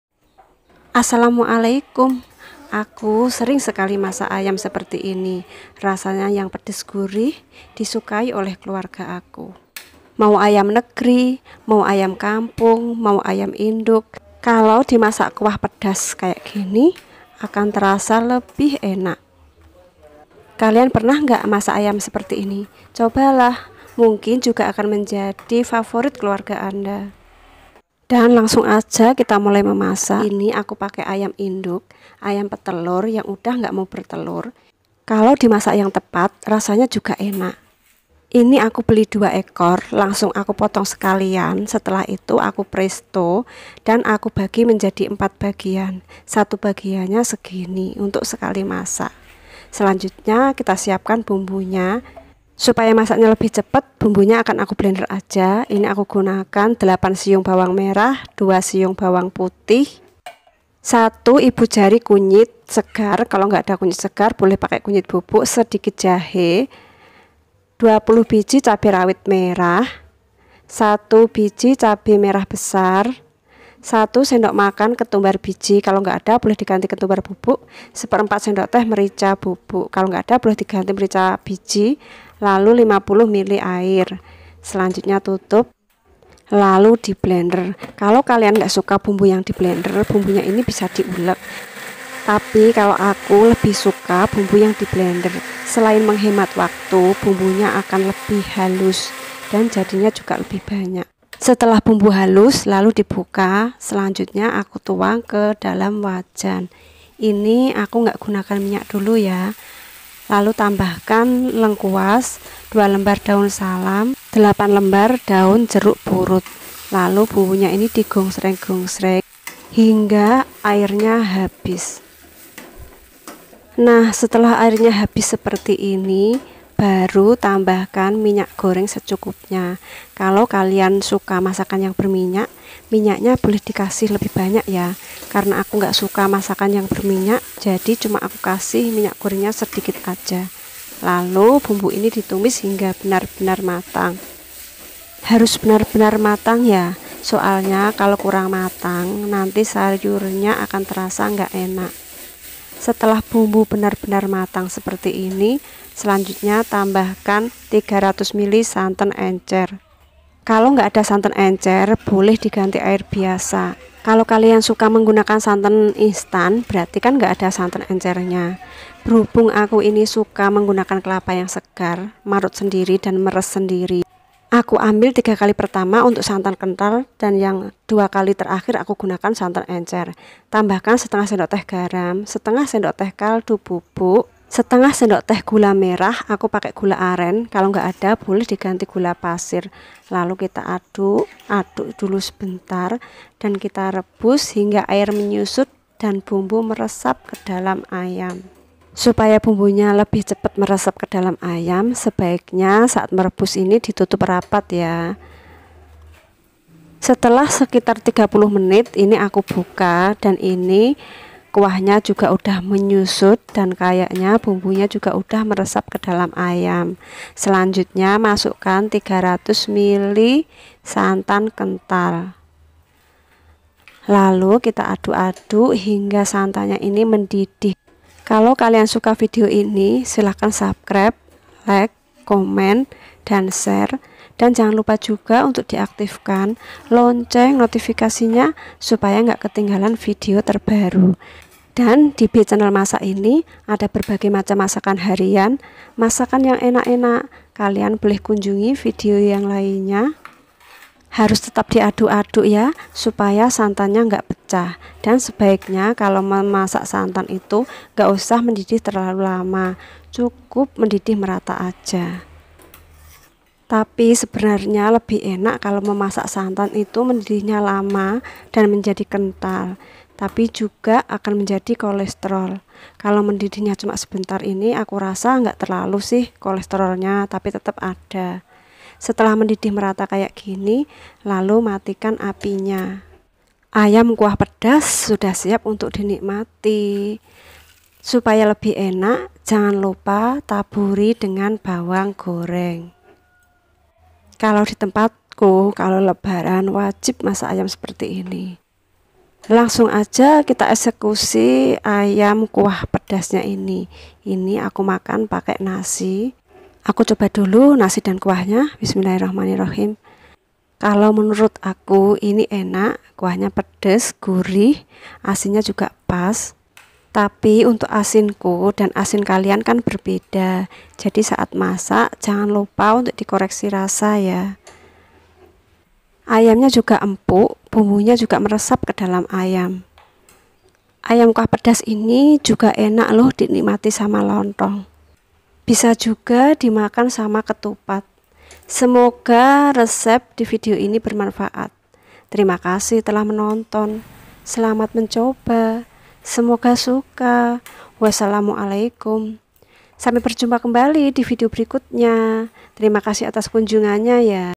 PEDAS GURIH, MANTUL ‼️RESEP AYAM KUAH PEDAS
Kali ini aku akan berbagi resep ayam kuah pedas,semoga kalian semua suka

Resep
1/2 ekor ayam induk
8 siung bawang merah
2 siung bawang putih
1 ibu jari kunyit
sedikit jahe
20 biji cabe rawit merah
1 biji cabe merah besar
1 sdm ketumbar biji
1/2 sdt merica bubuk
50 ml air
lengkuas
2 lembar daun salam
8 lembar daun jeruk purut
1/2 sdt garam
1/2 sdt kaldu bubuk
1/2 sdt gula merah
300 ml santan encer
300 ml santan kental
minyak goreng secukupnya
bawang goreng secukupnya